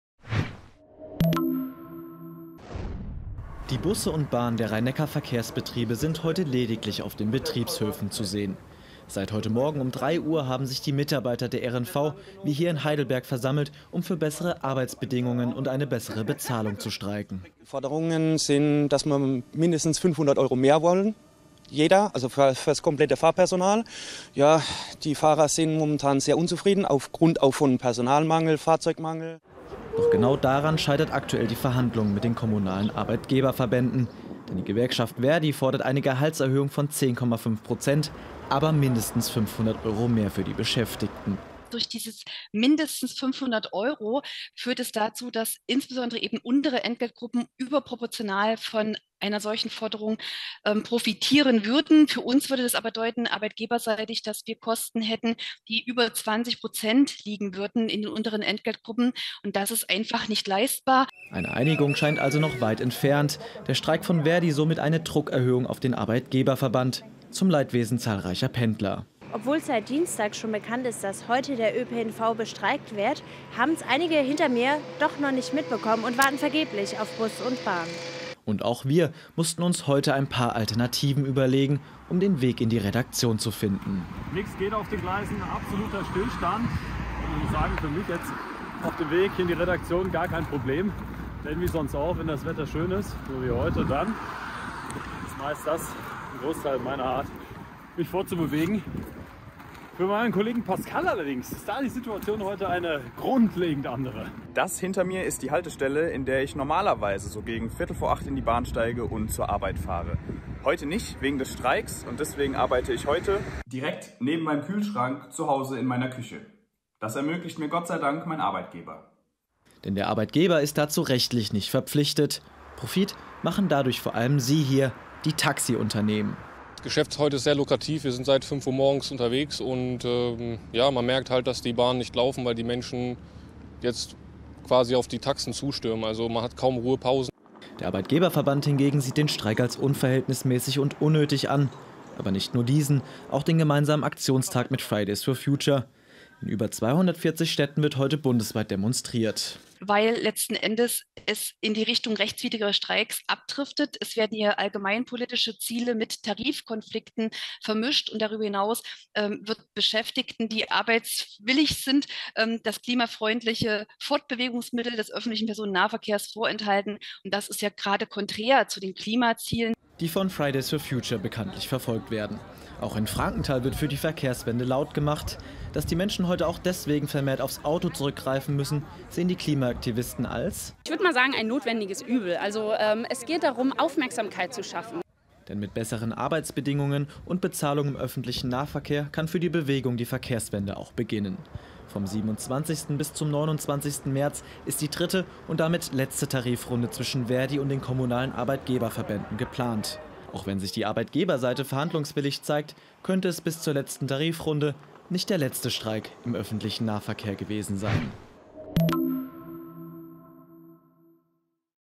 ÖPNV streikt mit Fridays for Future | RON TV
Vielen wird es spätestens heute Morgen aufgefallen sein. Busse und Bahnen stehen still. Auch wir in der Redaktion hatten mit dem Streik zu kämpfen. Außerdem haben auch die Klimaaktivisten Friday for Future demonstriert. Wir haben mit allen Beteiligten gesprochen und den Streiktag für euch zusammengefasst.